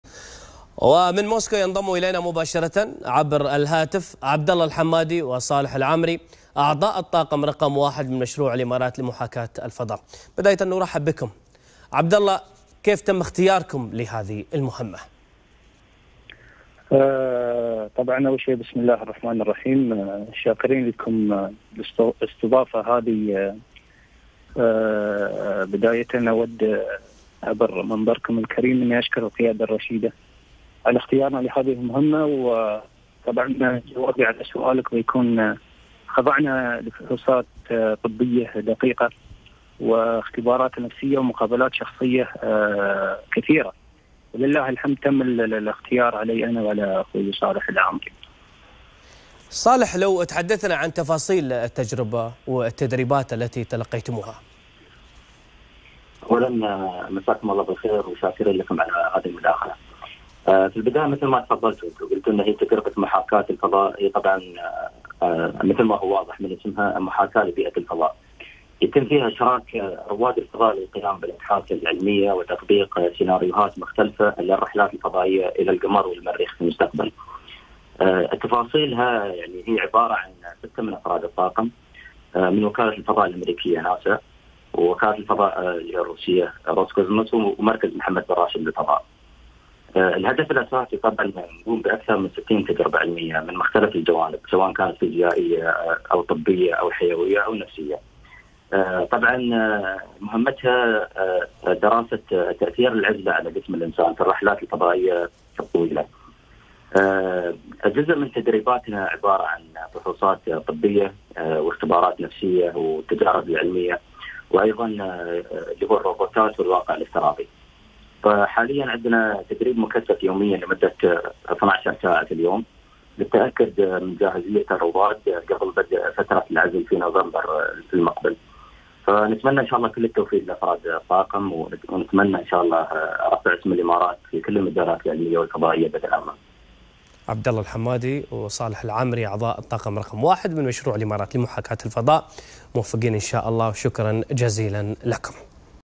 مداخلة هاتفية مع عبدالله الحمادي وصالح العامري عضوا الطاقم رقم واحد من مشروع الإمارات لمحاكاة الفضاء
أخبار الإمارات | مداخلة هاتفية مع عبدالله الحمادي وصالح العامري عضوا الطاقم رقم واحد من مشروع الإمارات لمحاكاة الفضاء